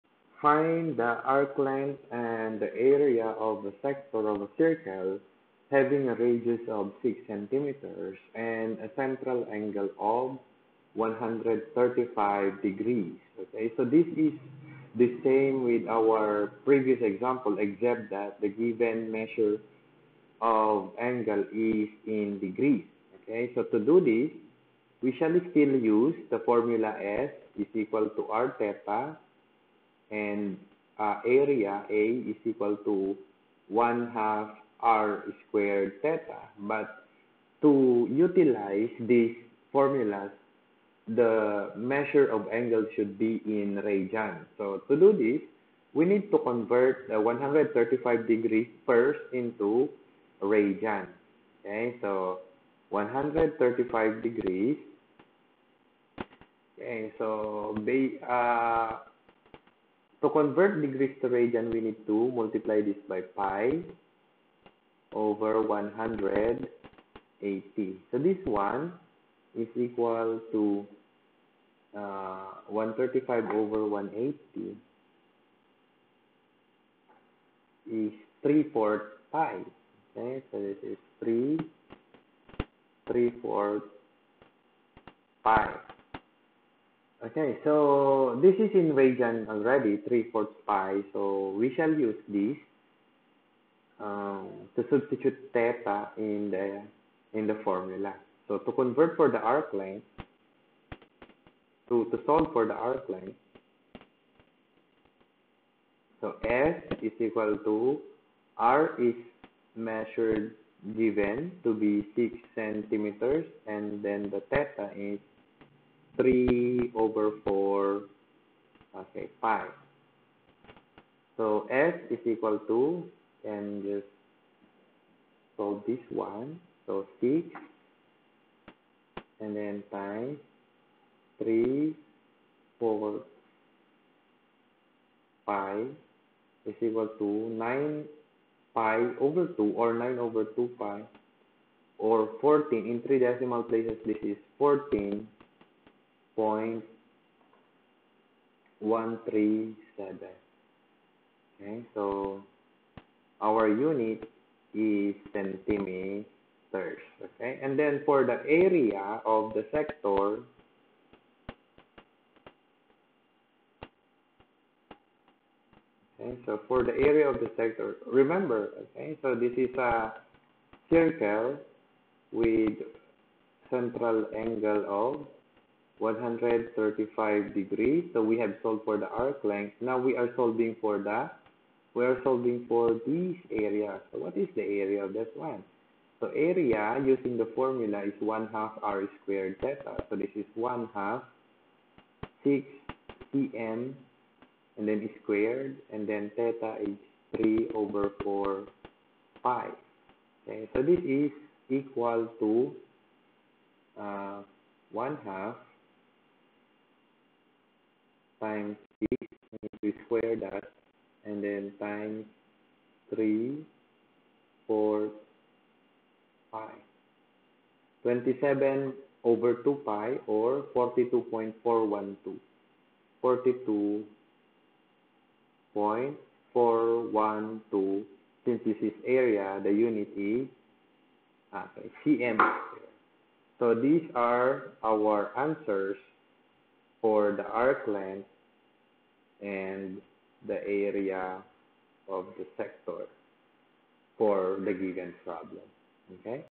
Solving for the Arc Length and Area of a Sector of a Circle
Course: ENGG10 Pre-calculus
Topic: Trigonometry
Sub-topic: Arc Length and Area of a Sector of a Circle
Question:
Find the arc length and the area of the sector of the circle having a radius of 6 cm, and a central angle of 135°.